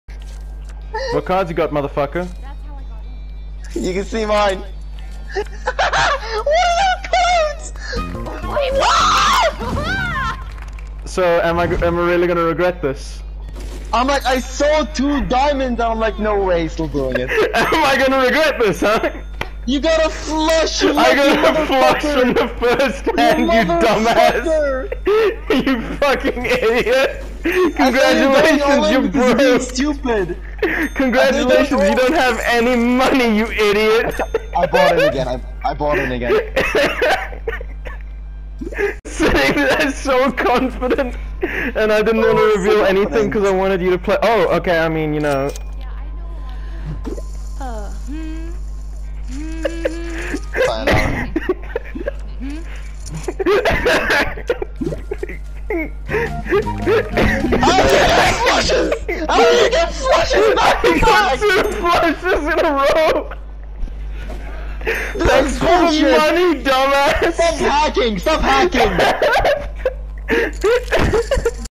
Blessed by Lady Luck
Yes, I know the optimal play on the second hand would be to bluff and bait him into betting higher, but I figured the comedic value was worth it to go all in